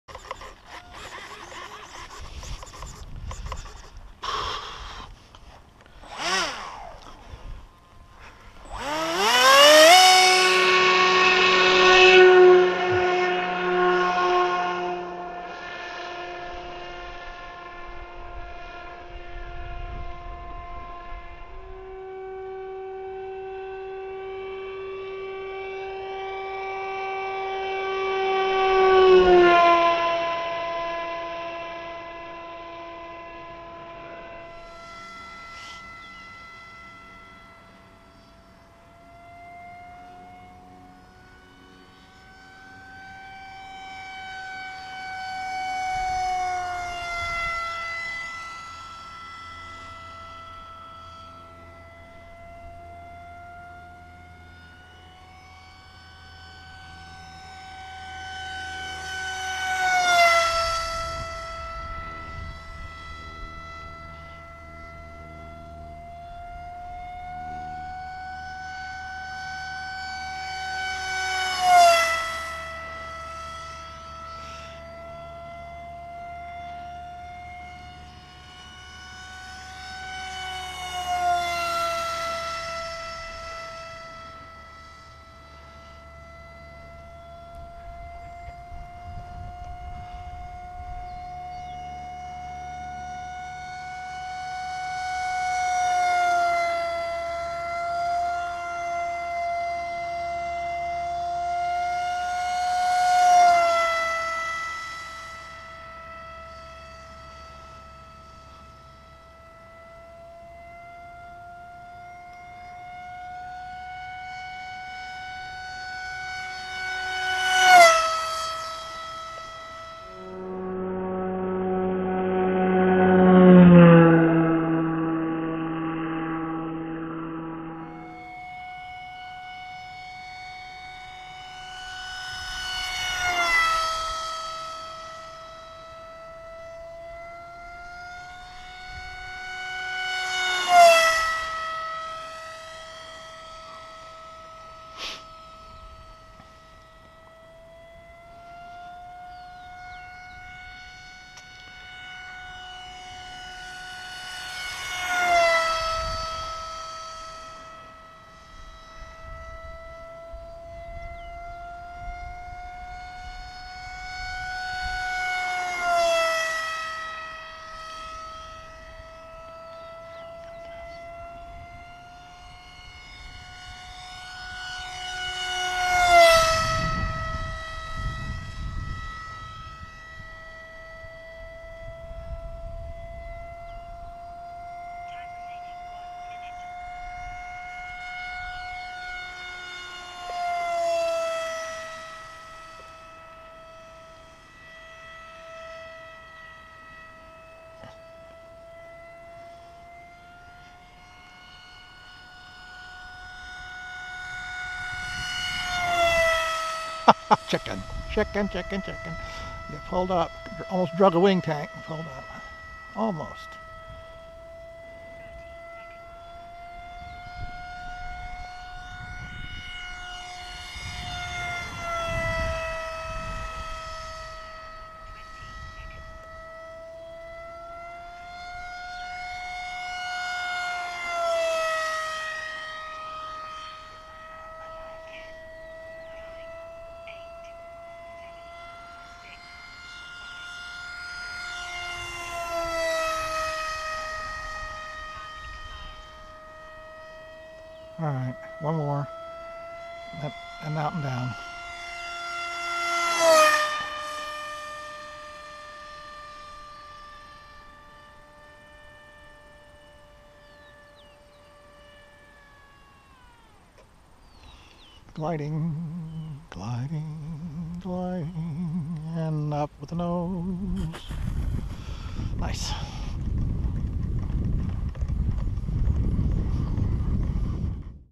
EVENING STARFIGHTER
The 5EZP F-104  STARFIGHTER TAKES TO THE EVENING SKIES.
I'm running desperately short of DOW blue fan-fold foam so I made the new forward fuse of my F-104 out of pink Formular fan-fold. Previously I reconfigured the wings but had the CoG off and mangled the fuse before I got the CoG dialed in.
The wing tanks are from my E-flite 64mm F-16.
Whereas most planes have a length to wing-span ratio of .6 (the golden ratio is (0.601) the prototype F-104's ratio is.43. The 5EZP F-104 has a ratio of .53.
P.S.
It rained most of there day today so no flying. With that I fixed the drooping fuse and painted the plane a soft gray. Thinking about a Luftwaffe Livery.